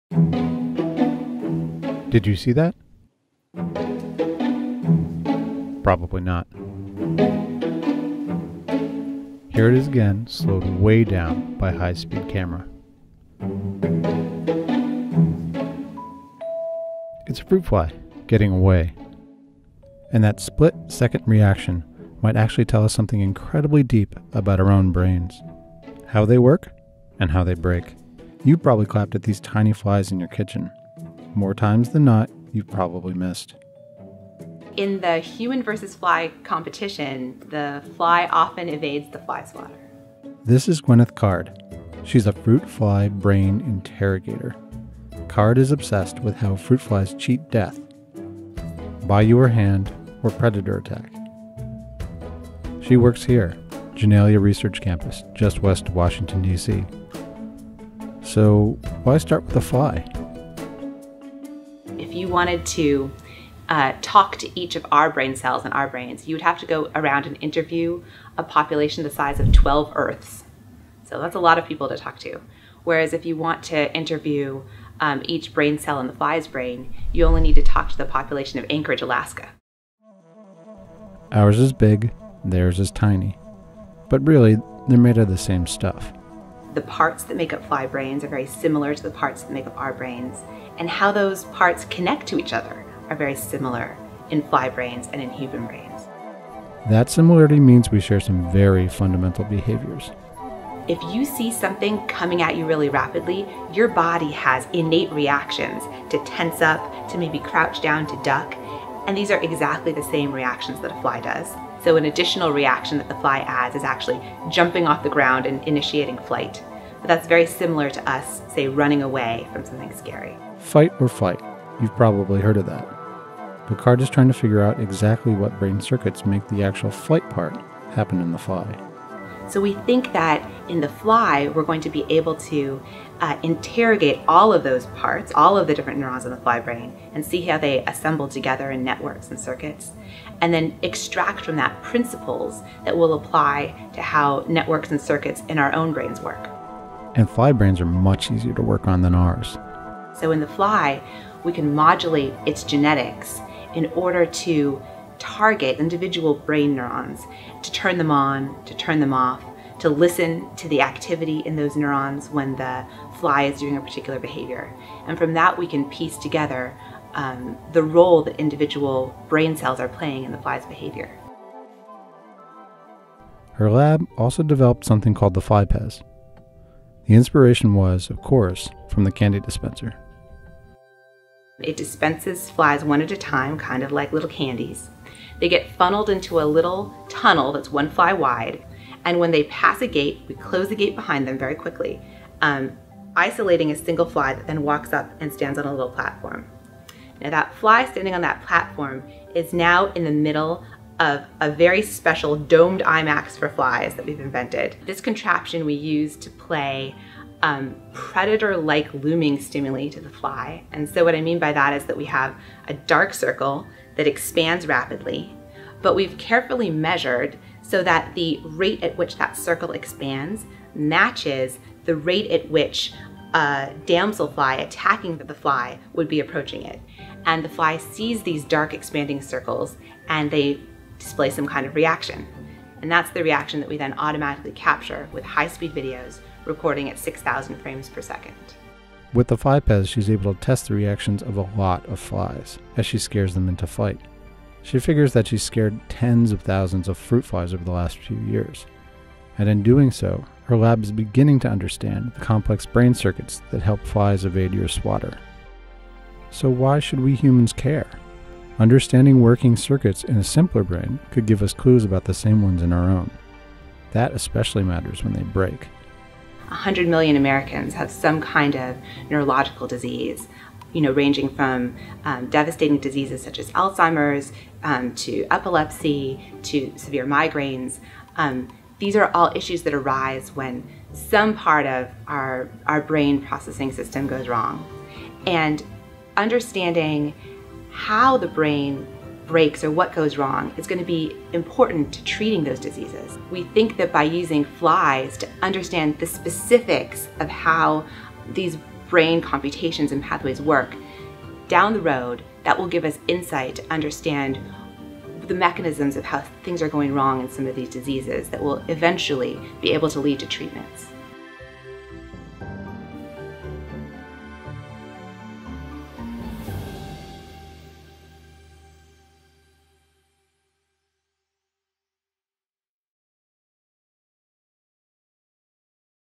She scares fruit flies for a living. Here’s why.
Gwyneth Card likes to say that she’s a “fruit fly liberator,” even if she frees her flies with the threat of death from above.